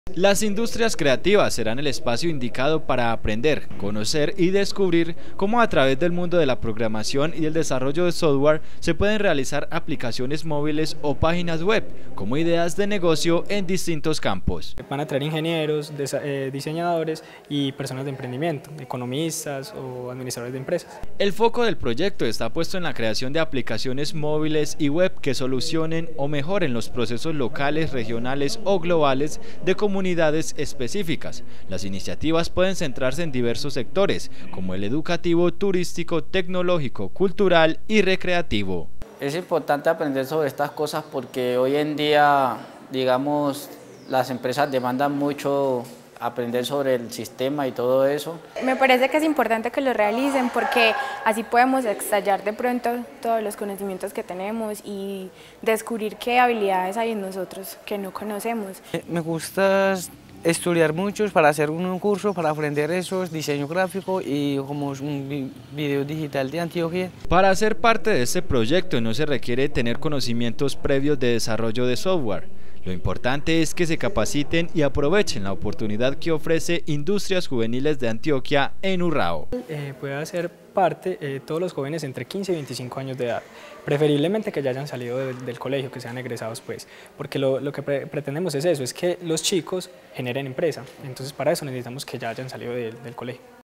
¿Quiéres ser parte de Industrias Juveniles?
Industrias juveniles es un programa de la Gobernación de Antioquia que busca incluir a mas jóvenes en el mercado laboral por medio de la creación de empresas desarrollando aplicaciones y software.